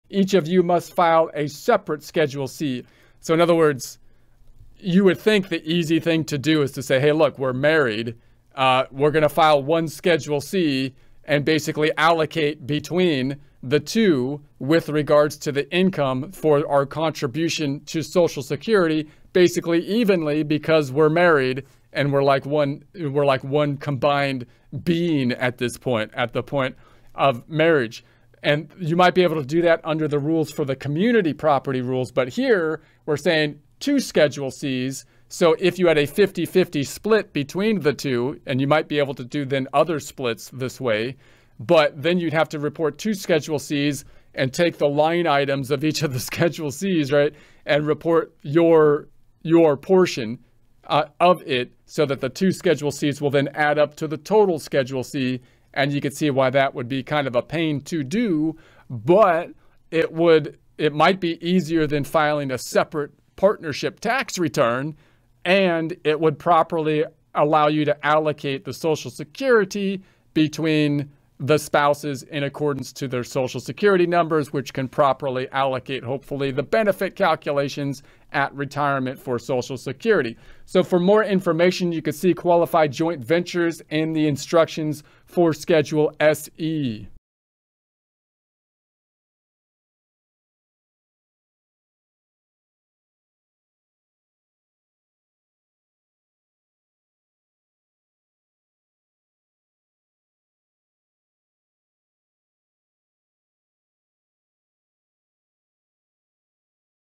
Recap of the challenges and options for spouses in business Tax 2023-2024
"Explore the dynamic landscape of spouses in business and the tax challenges they face in the tax year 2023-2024. Our latest video offers a comprehensive recap of the hurdles and options available to couples running businesses together. From navigating joint ownership complexities to maximizing tax benefits, we dissect the strategies and considerations essential for success. Whether you're embarking on a joint venture or seeking to optimize your current business structure, this recap provides valuable insights to help you thrive. Don't miss out – watch now and conquer the challenges of business ownership as spouses!"